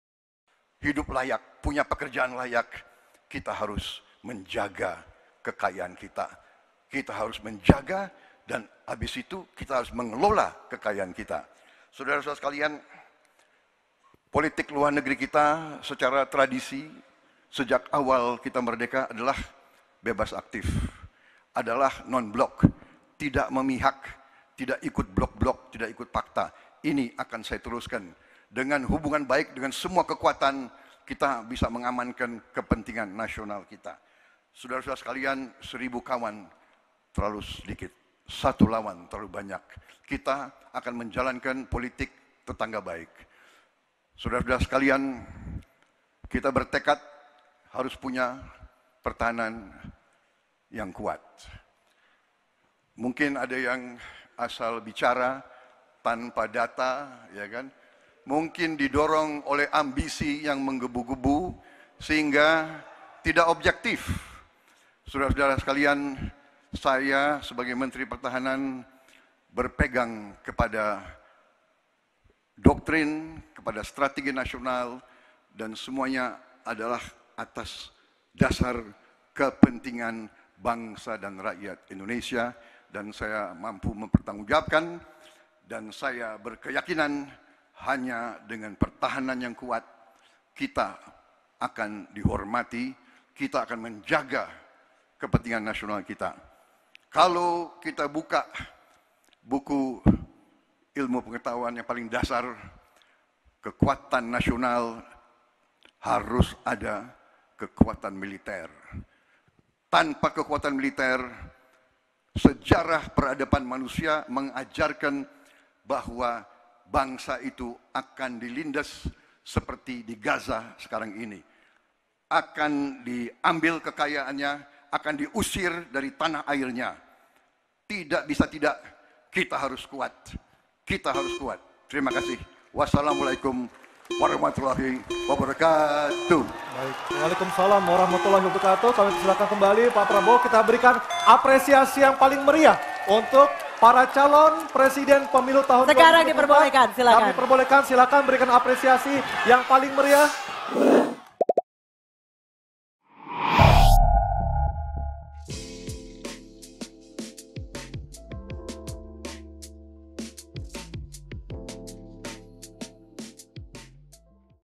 Prabowo Lempar Sindiran di Awal Debat Ada yang Asal Bicara Didorong Ambisi!
MERDEKA.COM - Capres Prabowo Subianto menyinggung pihak asal bicara soal pertahanan demi ambisinya. Menurut Prabowo, pihak tersebut berbicara tanpa data dan tidak sesuai dengan realita.

Debat capres berlangsung di Istora Senayan, Jakarta, Minggu (7/1). Tema debat kali ini yaitu Pertahanan, Keamanan, Hubungan Internasional, dan Geopolitik.